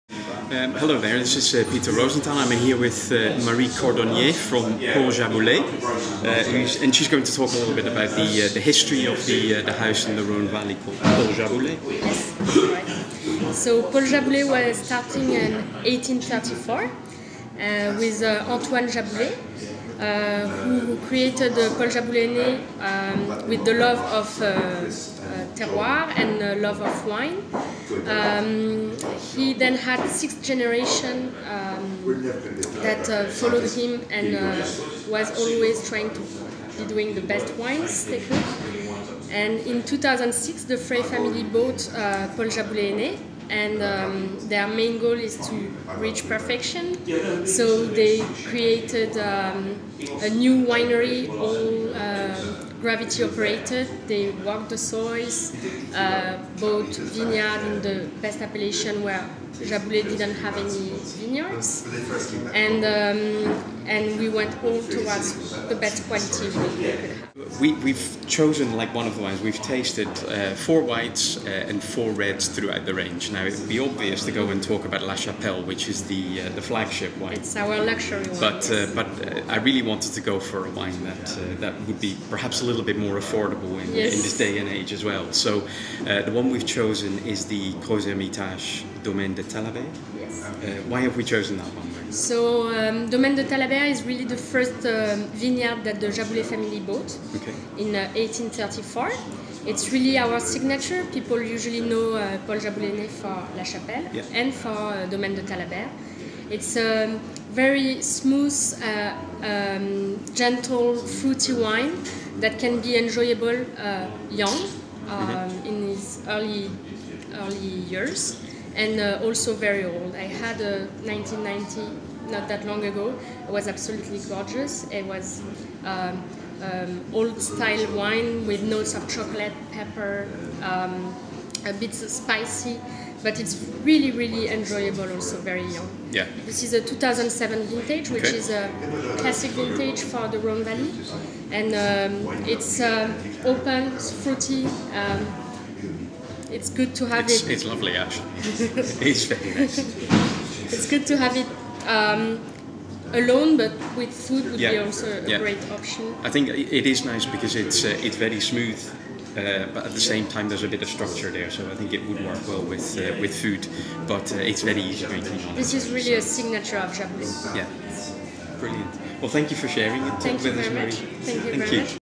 Paul Jaboulet Wine Tasting
Pieter Rosenthal tastes the wines of Rhone valley wine producer Paul Jaboulet with Marie Cordonnier. We choose to focus on Domaine de Thalabert Crozes Hermitage, which is the original vineyard of the domaine.